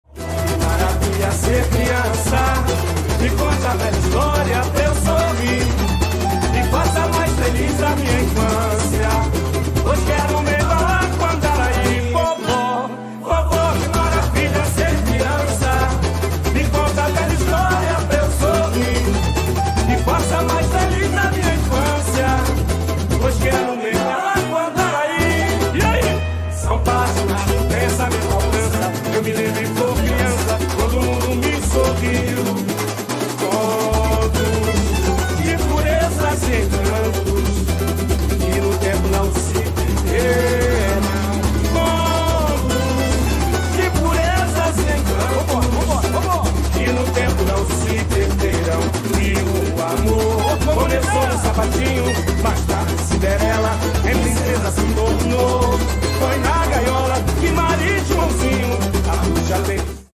Carnaval de Vitória 2024 - Andaraí
Debaixo de muita chuva, Andaraí abre 1ª noite de desfiles contando as histórias da vovó.